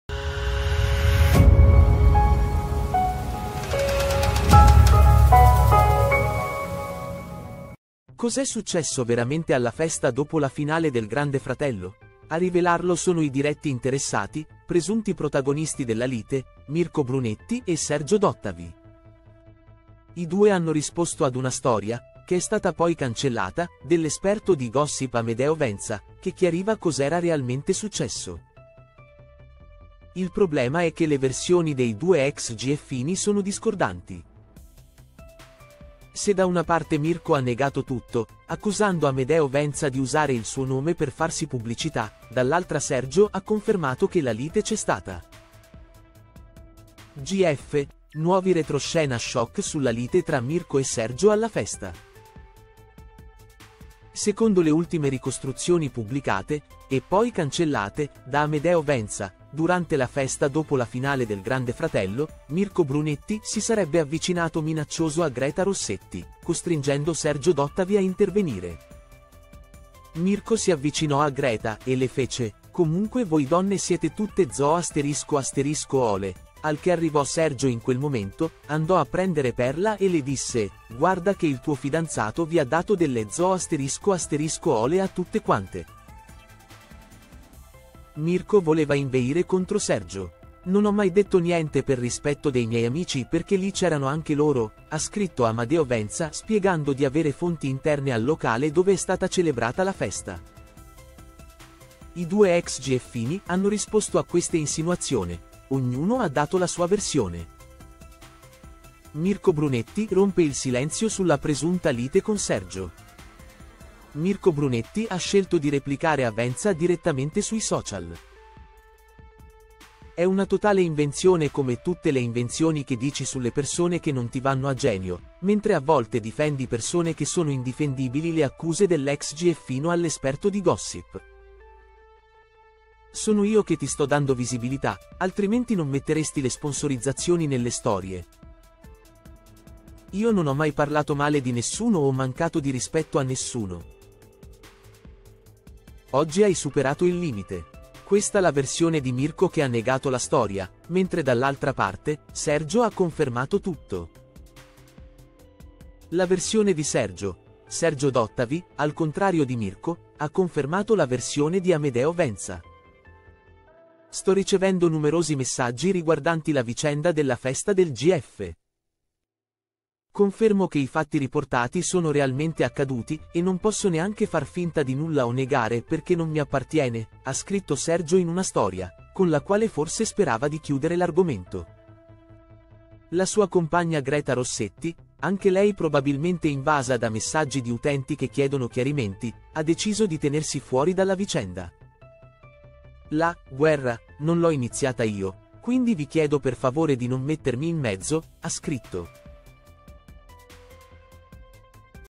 “GF”, nuovi retroscena choc sulla lite alla festa Mirko nega, Sergio conferma
Cos’è successo veramente alla festa dopo la finale del Grande Fratello? A rivelarlo sono i diretti interessati, presunti protagonisti della lite: Mirko Brunetti e Sergio D’Ottavi. I due hanno risposto ad una storia (che è stata poi cancellata) dell’esperto di gossip Amedeo Venza, che chiariva cos’era realmente successo. Il problema è che le versioni dei due ex gieffini sono discordanti. Se da una parte Mirko ha negato tutto, accusando Amedeo Venza di usare il suo nome per farsi pubblicità, dall’altra Sergio ha confermato che la lite c’è stata.